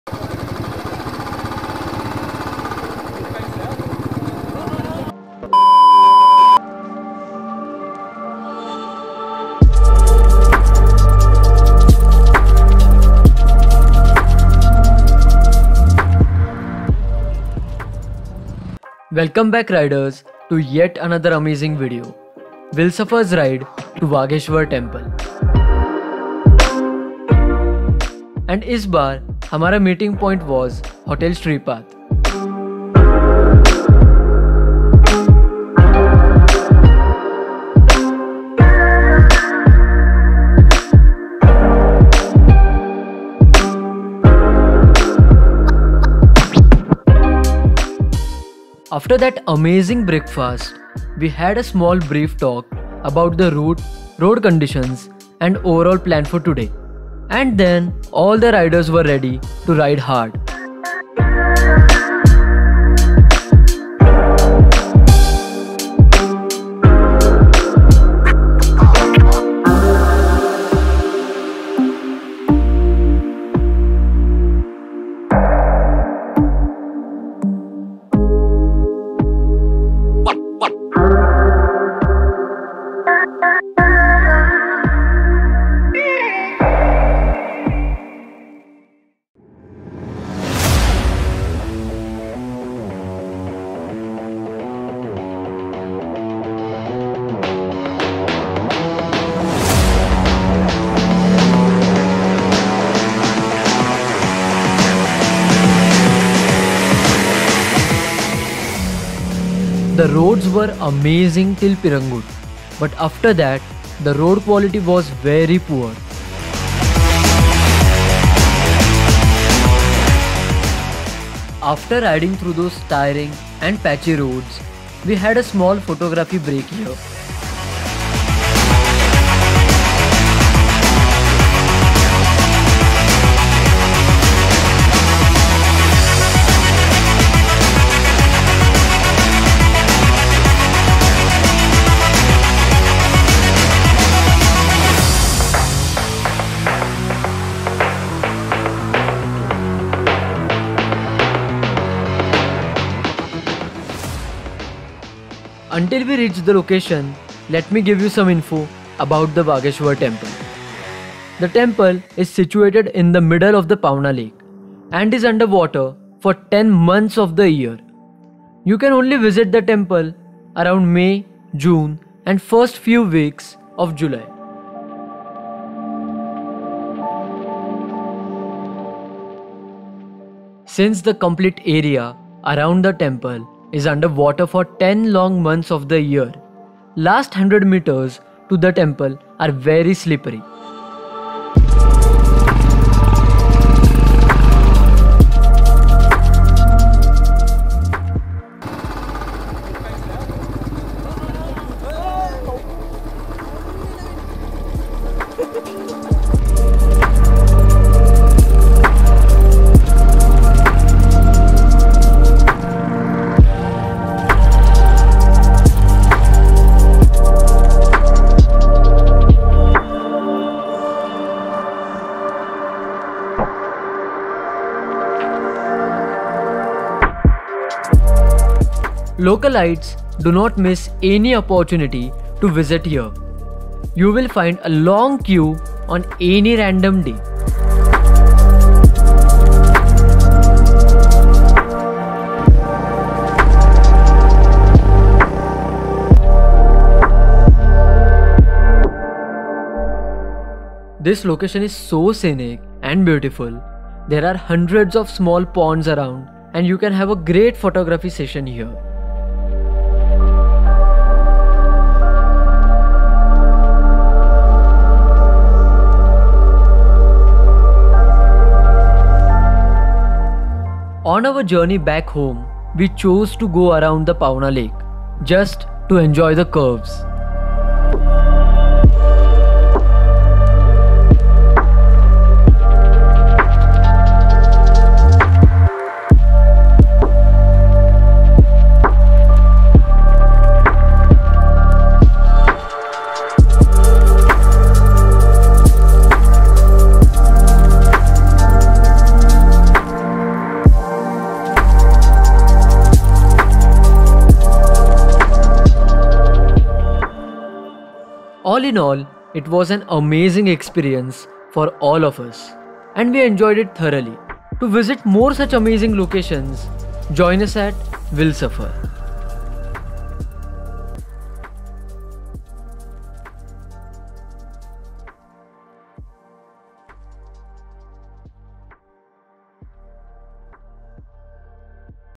Stuck In The Muds Of Wagheshwar? Under Water Temple | WheelSafar | Riding Community | Pune
We hope that you liked our Ride To Wagheshwar.